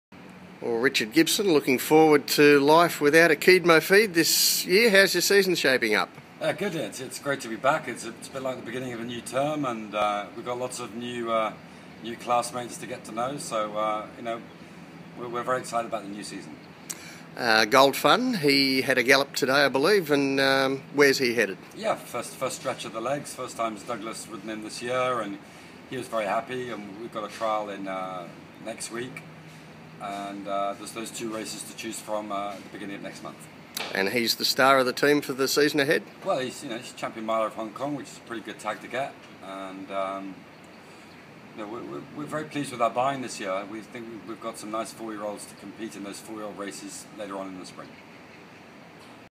Richard Gibson - Pre-season Thoughts
Trainer Richard Gibson talks about life after stable star Akeed Mofeed.